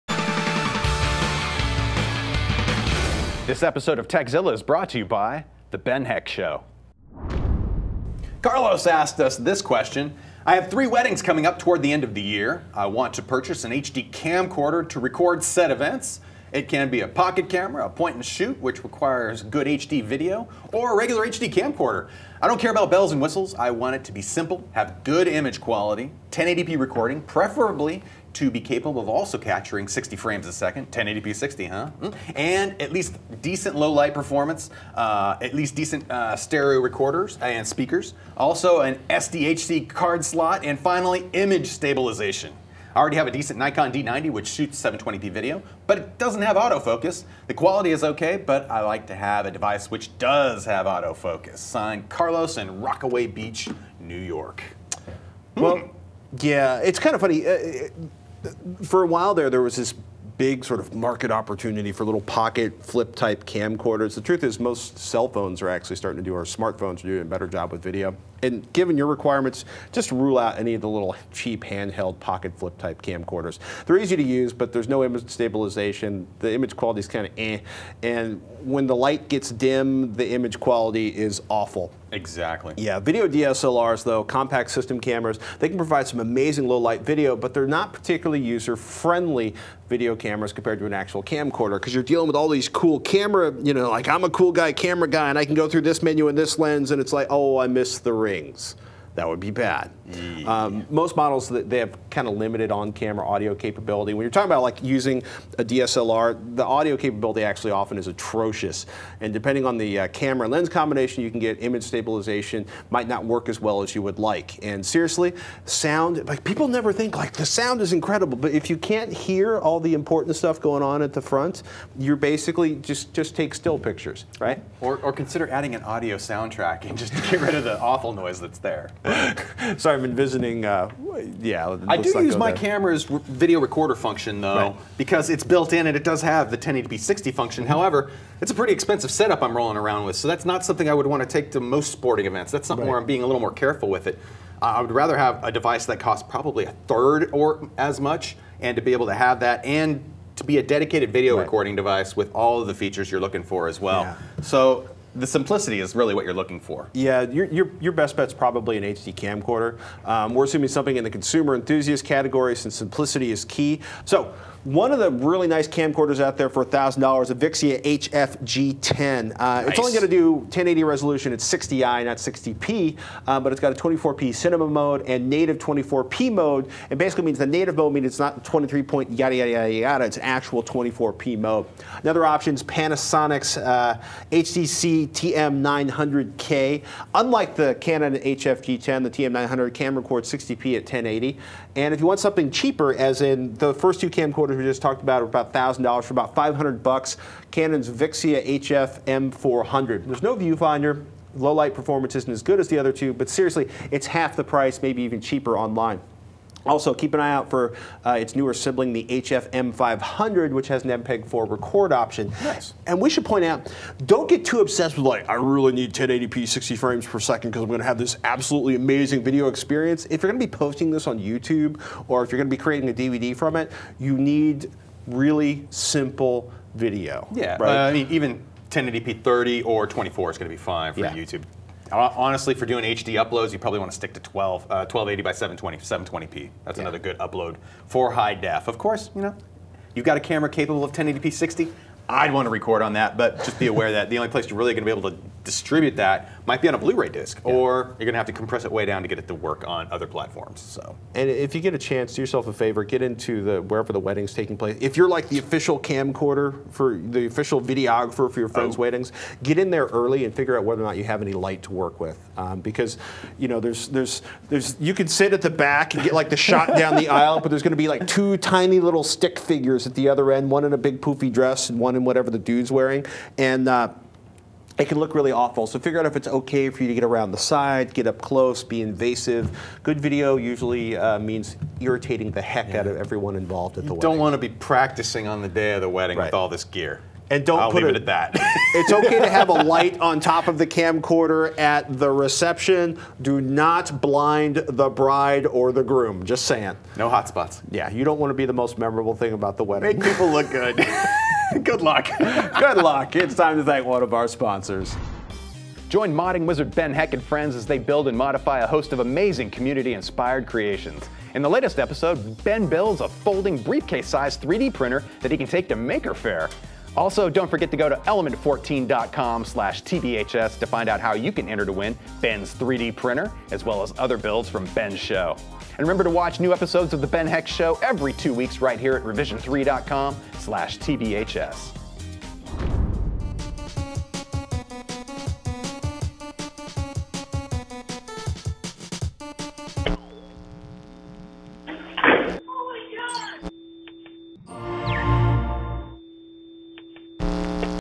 3 Great HD Camcorders For Weddings!
Carlos has 3 weddings coming up towards the end of this year and wants to purchase an HD Camcorder to record them. We've got three great HD camcorder recommendations, some tips on wedding videography and why we wouldn't bother with a pocket camera, and why DSLRs might not be the best option for many budding videographers!